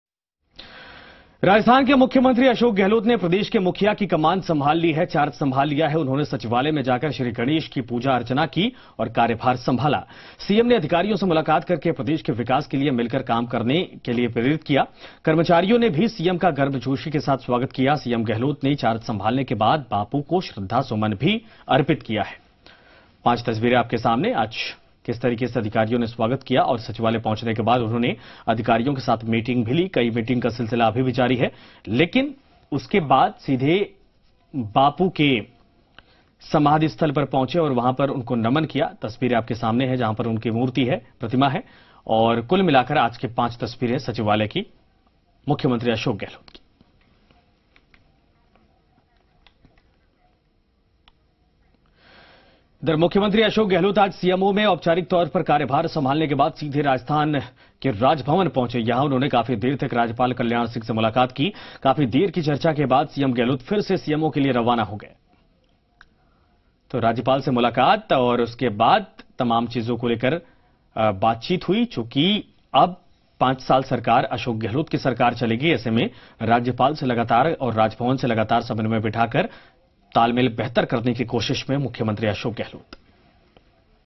जोधपुर: आतंकवाद से लड़ने के लिए भारत-रूस की वायु सेना कर रही है युद्धाभ्यास
सचिवालय में सीएम गहलोत ने की पूजा-अर्चना, प्रदेश के विकास के लिए अफसरों में भरा जोश